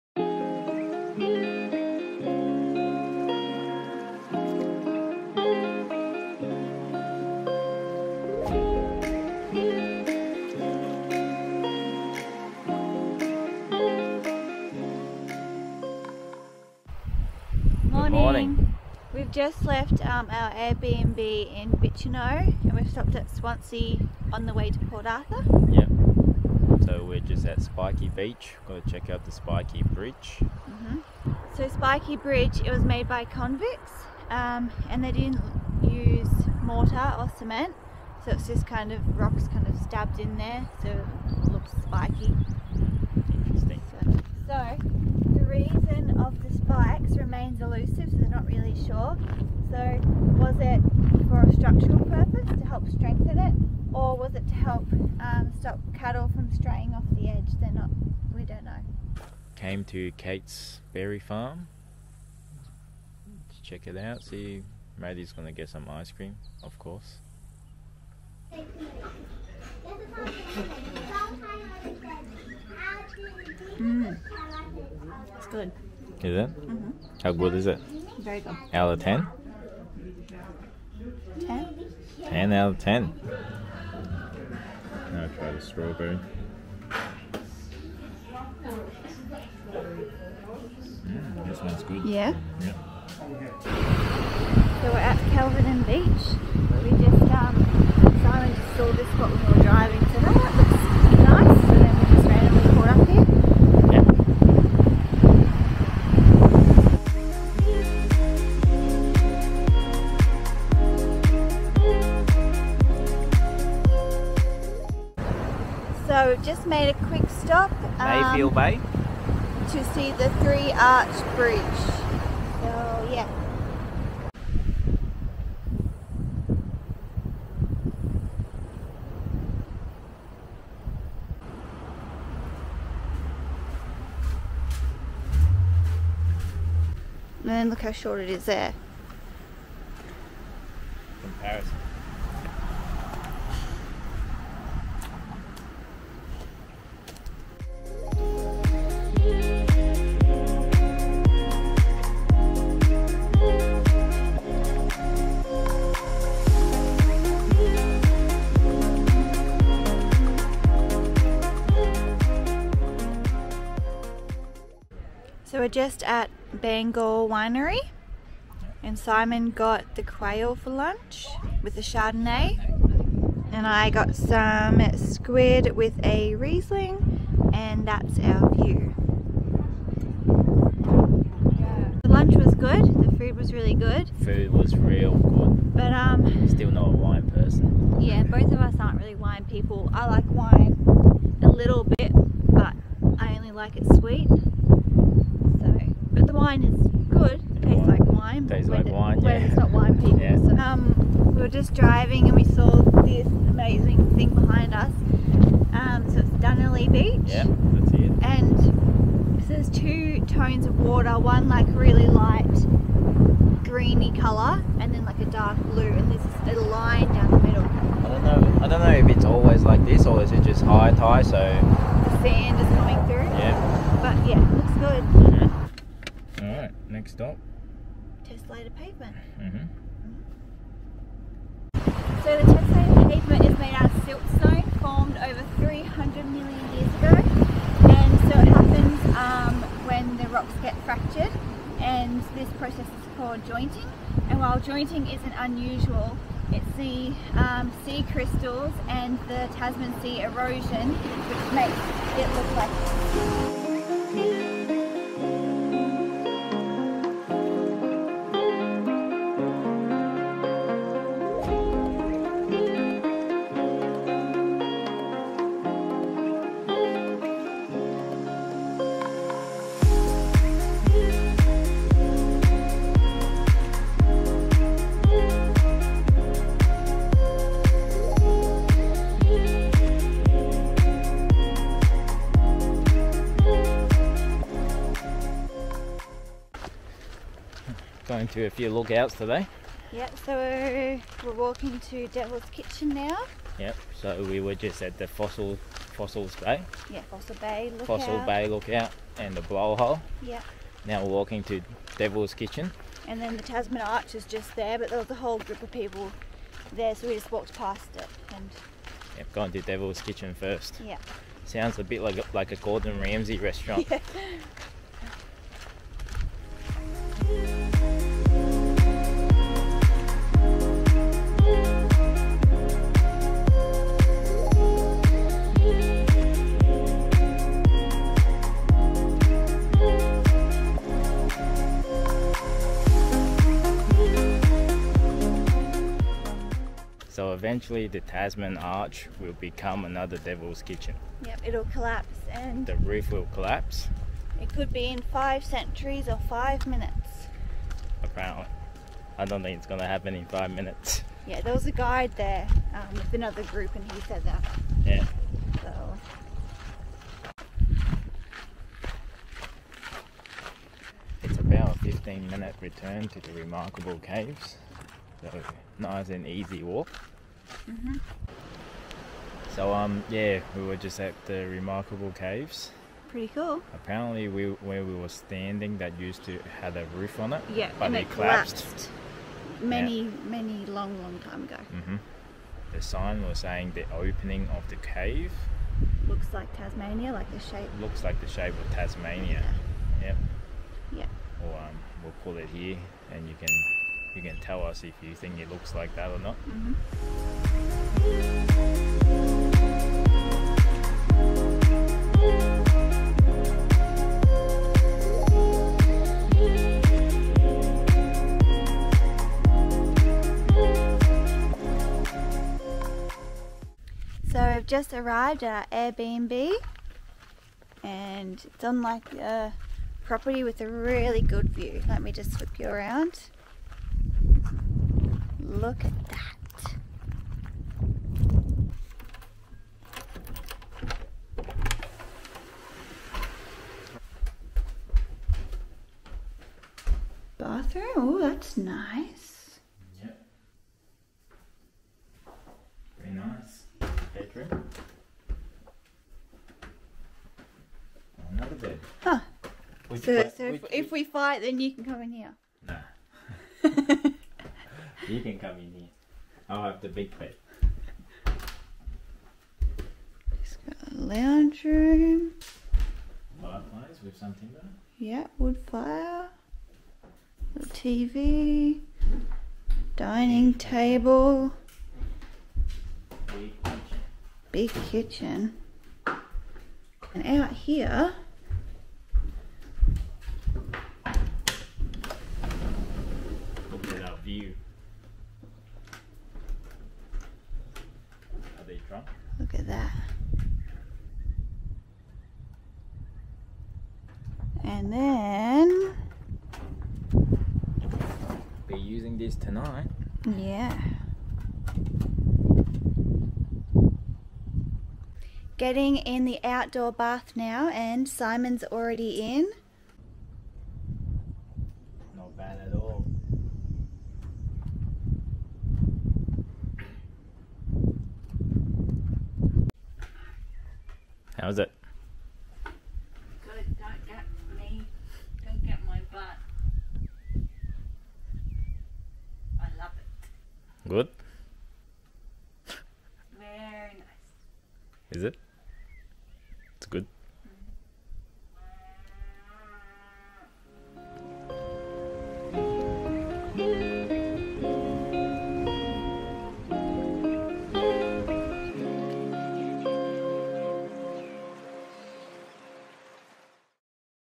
Bicheno to Port Arthur | Tasmania Vlog 6
Today we travelled from Bicheno to Port Arthur stopping along the way to admire the magnificent beauty of both natural and man-made wonders.

Our first stop is Spiky Bridge, located in the town of Swansea. This unique and historic bridge was built by convicts in 1843 and is made entirely of local fieldstone.

We then spontaneously stopped at Dunnely beach as the incredible contrasting colours of the water gave us no choice but to stop!

We visited Devil's Kitchen, a stunning coastal rock formation, Remarkable caves carved out of the coastal cliffs and the Tasman arch, another natural wonder located in the Tasman National Park. We also explored
Tessellated Pavement, an incredible geological wonder made up of a series of flat, rectangular slabs that resemble a tiled floor. Created by the power of erosion, these places are definitely sights to behold!

And of course we had to stop at a winery- it's what Tasmania is famous for!